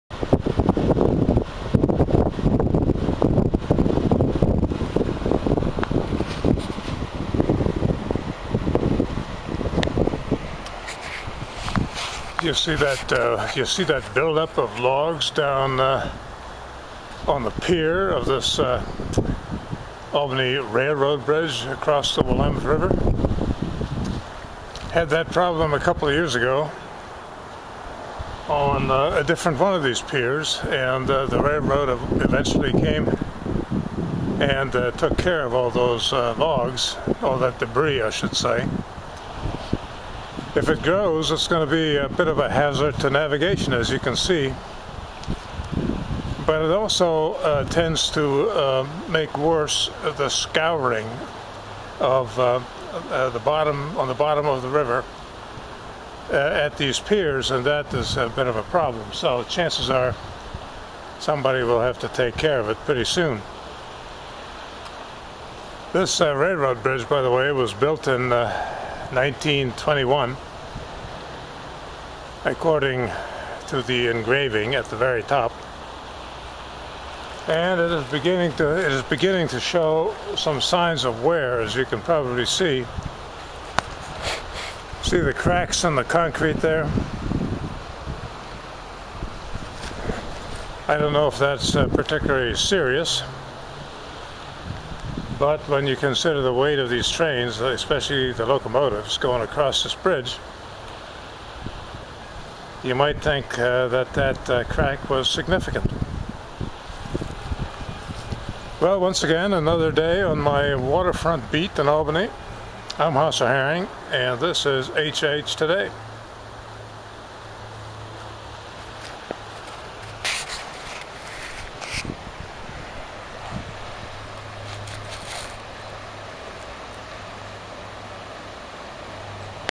100 0417
A railroad bridge shows some wear ...